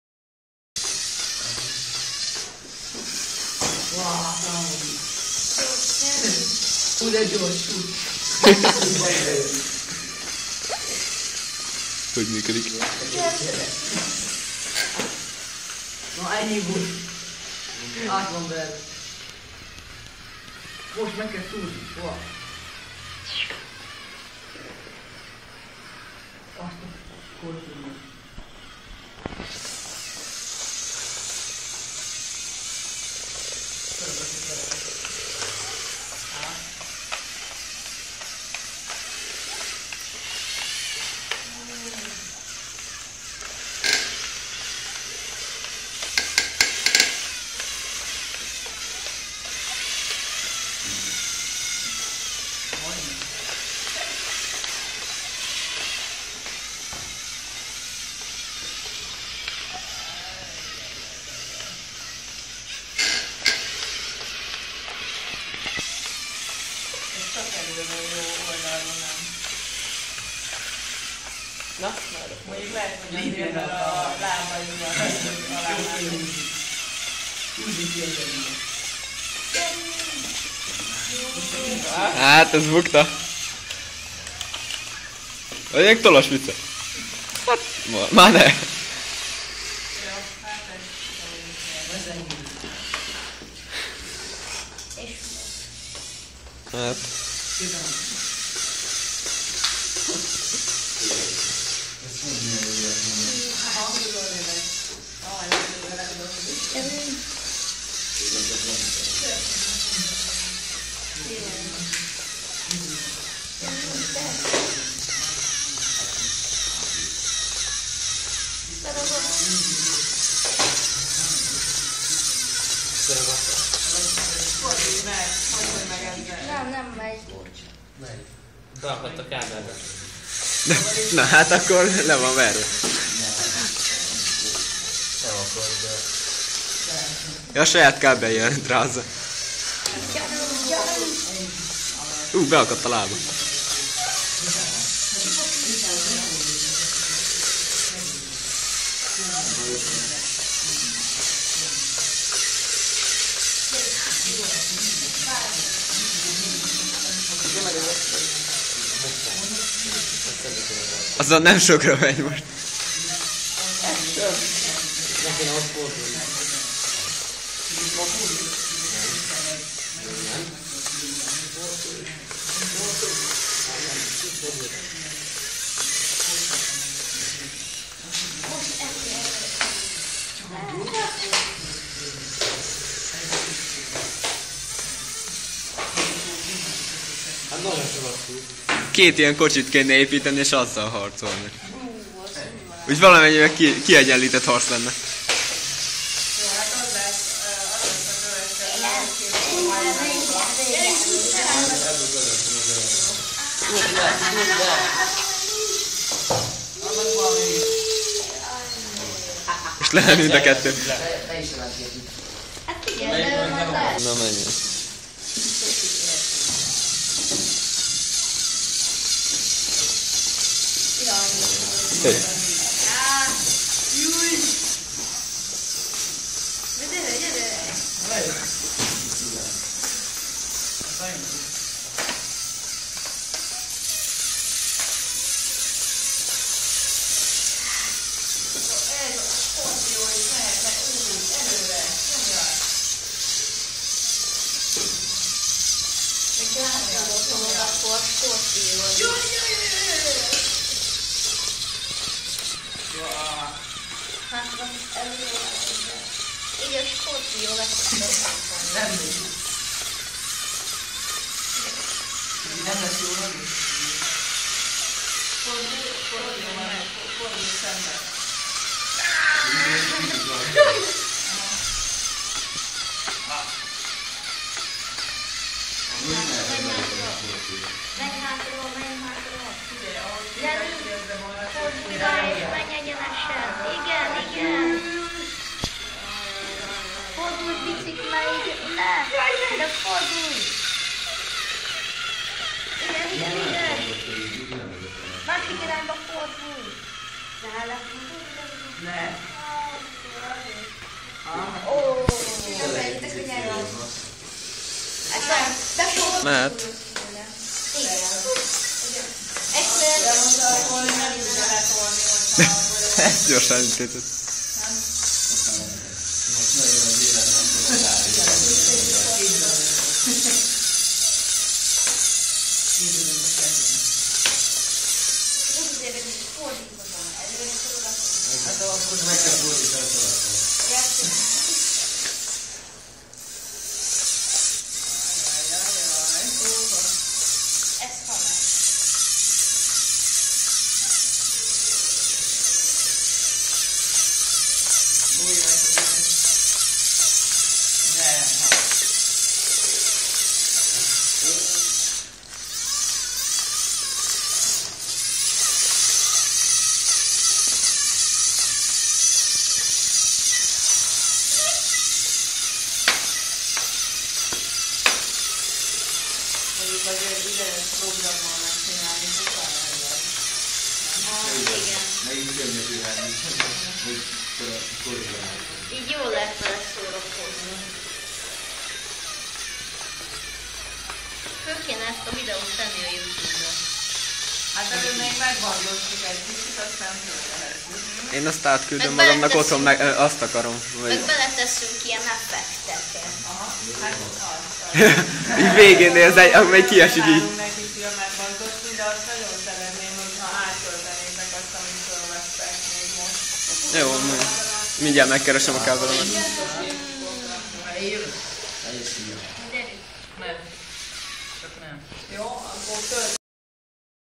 Lego Mindstorm Fight
Déri Miksa Szegedi Iskolában Számítástechnikai Szakkörön ezeket építettük és Androidos Telefonnal irányítottuk a gépeket.